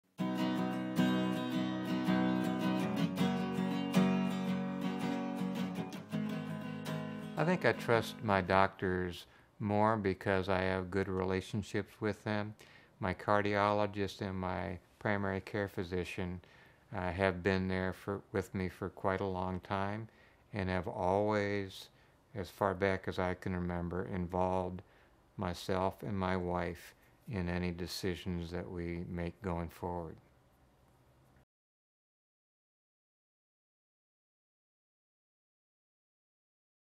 Choosing Wisely: The Importance of Shared Decision-Making (Stephen Hanson)
Stephen Hanson, a two-time stroke survivor speaks about the importance of shared decision-making between himself, his wife, and his physicians regarding his treatment.

The main principle of ABIM Foundation and Choosing Wisely is patient-centered care and the promotion of physician/patient conversations.